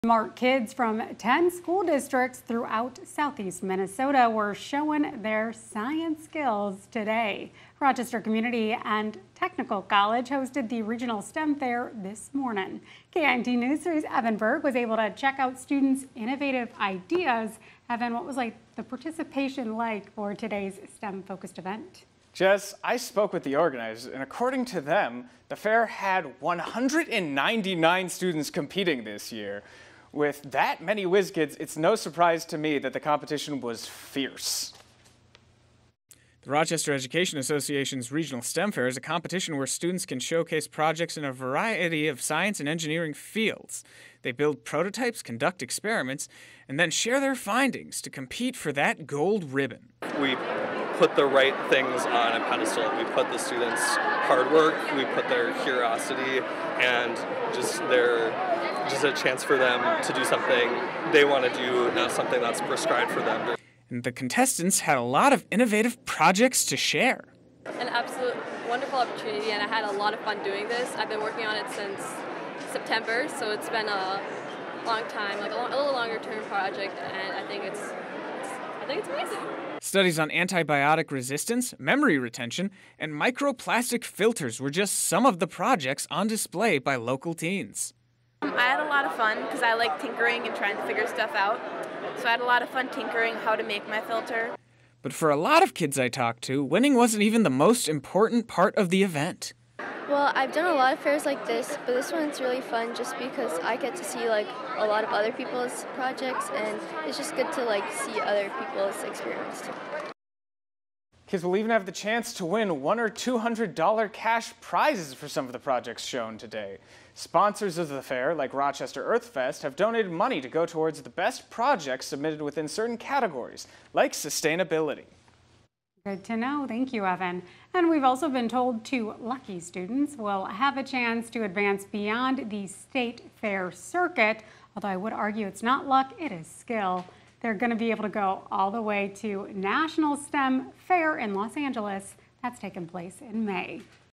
Whiz kids show their science skills at Rochester regional STEM fair
Some skilled students could even win a prize for their projects.